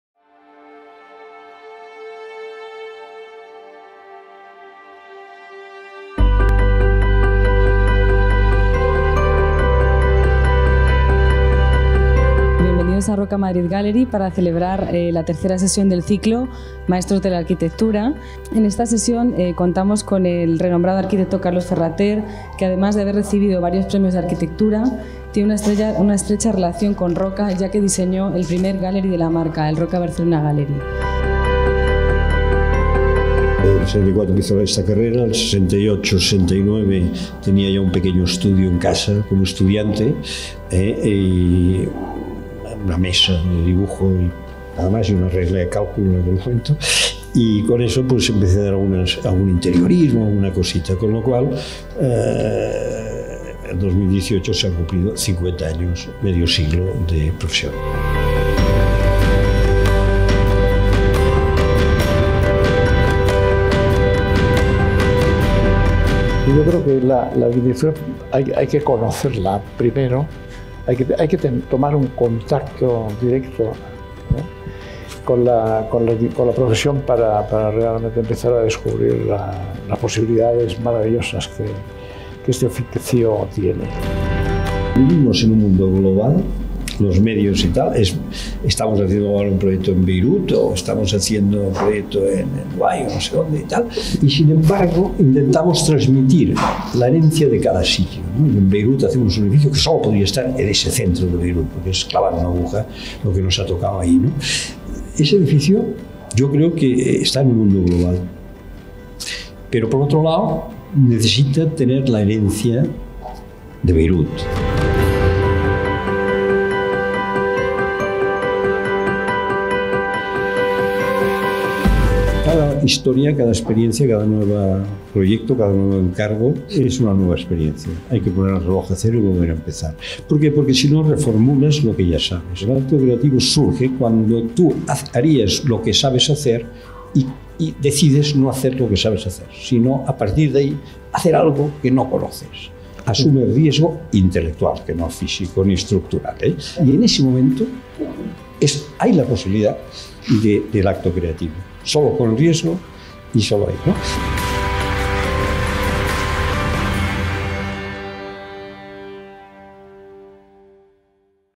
Maestros de la Arquitectura Carlos Ferrater I Roca Madrid Gallery
En esta sesión conversamos con el arquitecto Carlos Ferrater

Roca Madrid Gallery celebró una nueva sesión del ciclo de entrevistas ‘Maestros de la arquitectura’. Gracias a este ciclo se da a conocer la parte más humana e inspiracional que se esconde tras la figura del profesional. Santiago Fajardo, comisario del ciclo, nos presentó en esta ocasión al arquitecto Carlos Ferrater. Nacido en Barcelona, es doctor arquitecto y catedrático de Proyectos Arquitectónicos de la UPC y director de la Cátedra Blanca de Barcelona.

En el año 2006 constituye con Xavier Martí, Lucía Ferrater y Borja Ferrater la sociedad Office of Architecture in Barcelona (OAB), con Núria Ayala como Directora de Proyectos.

Premio Nacional de Arquitectura 2009 por su trayectoria, otorgado por el Ministerio de Vivienda y nombrado desde diciembre de 2011 Miembro del Royal Institute of British Architects (International Fellow of the RIBA).
In this session we talked with the architect Carlos Ferrater

Roca Madrid Gallery celebrated a new session of the ‘Masters of architecture’ interviews cycle. Thanks to this cycle we’ll get to see the most human and inspirational part that hides behind the figure of the professional. Santiago Fajardo, commissioner of the cycle, presented, on this occasion, the architect Carlos Ferrater. Born in Barcelona, ​​he is a doctoral architect and professor of Architectural Projects at the UPC and director of the ‘Cátedra Blanca’ of Barcelona.

In 2006, with Xavier Martí, Lucía Ferrater and Borja Ferrater, he formed the Office of Architecture in Barcelona (OAB), with Núria Ayala as Project Director. For his career, he was awarded by the Ministry of Housing the National Architecture Award 2009 and appointed since December 2011 Member of the Royal Institute of British Architects (International Fellow of the RIBA).